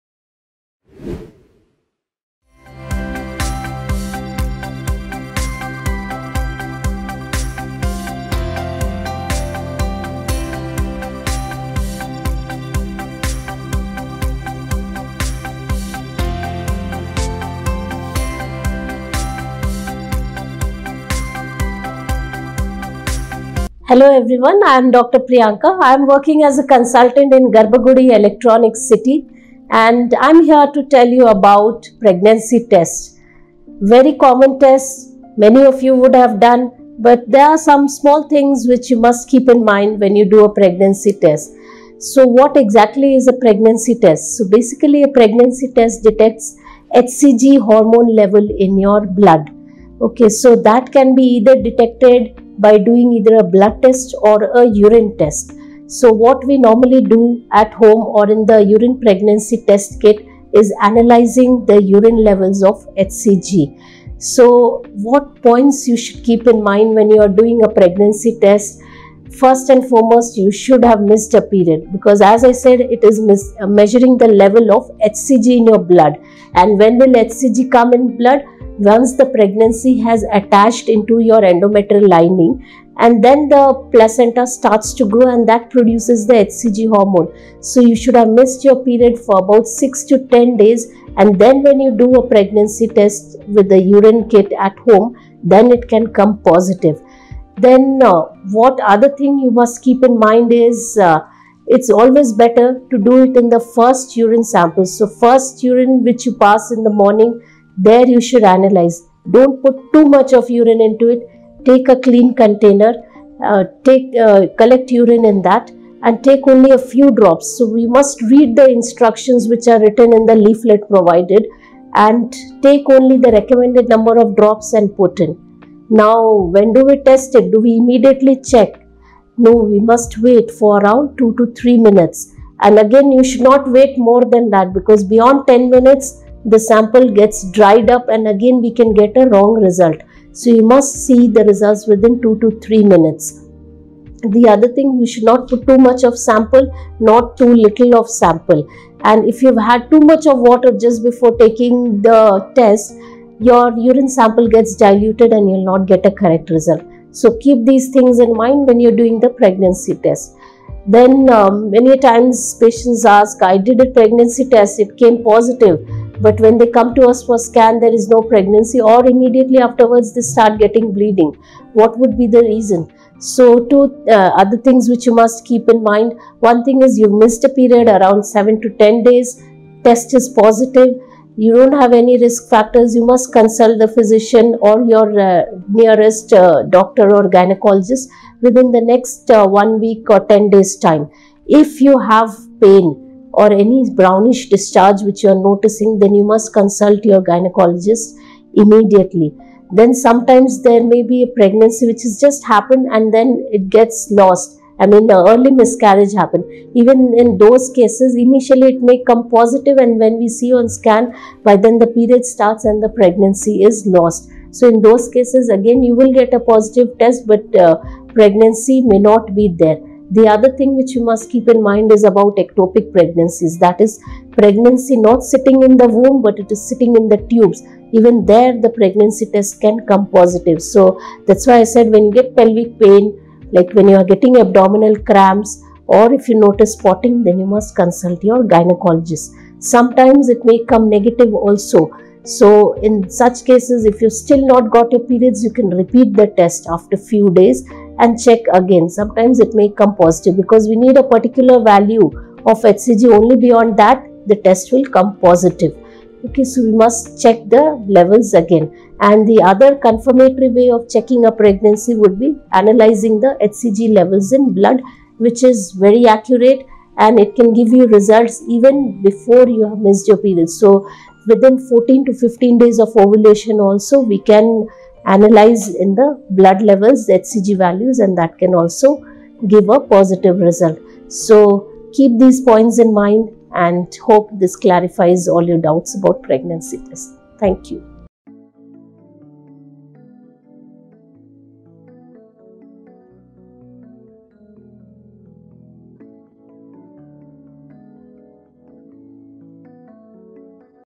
Urine Pregnancy Test - Dr. Priyanka Rani
Today we have Dr. Priyanka, Fertility Specialist at GarbhaGudi IVF Centre, here to explain the process of taking a urine pregnancy test. She will go over how the test works when to take it, and what to expect with the results. Whether you are trying to conceive or just want to know if you are pregnant, this video will provide all the information you need.